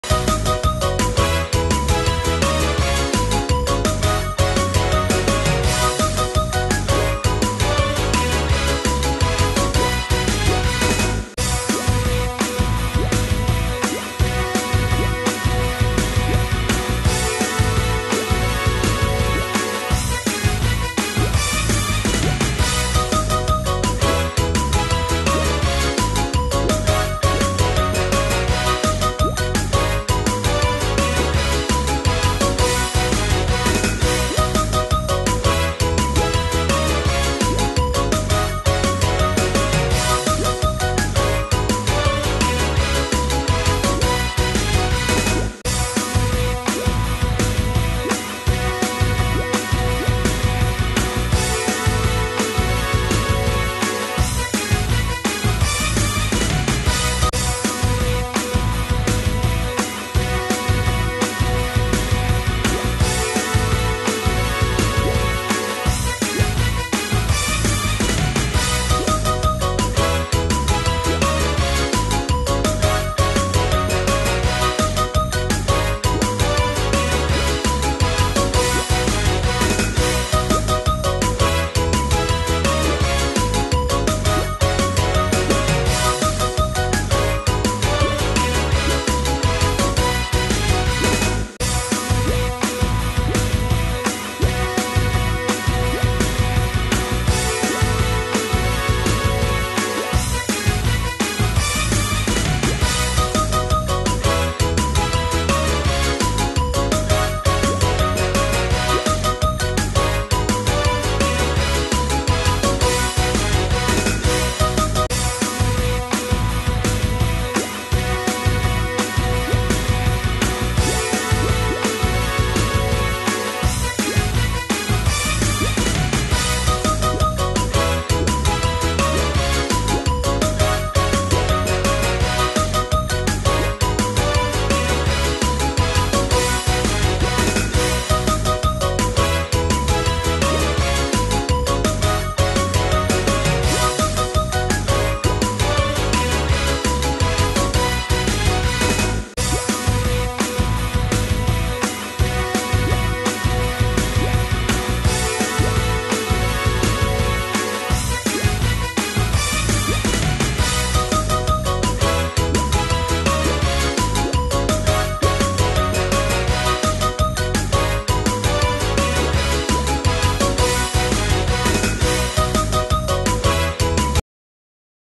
Athletic girl dress up game
Do you like sports and exercises at the gym? And what you weaer for the exercises? Let's help this girl to choose an outfit for the sport.